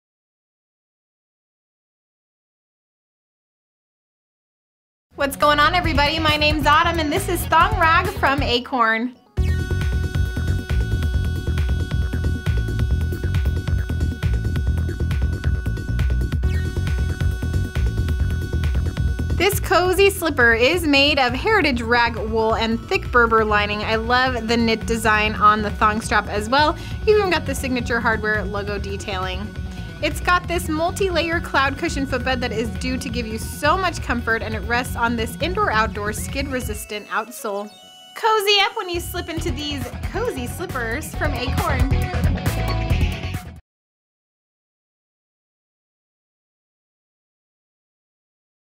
Giày Acorn Thong Ragg
Giày bấm mua ngay tại Thong Ragg giống như đang mặc một đám mây! Được xây dựng bằng một hỗn hợp len ragg di sản với lớp lót Berber dày. Nhiều lớp mây Cushion ™ footbed. Đế ngoài chống trượt ngoài trời / trong nhà.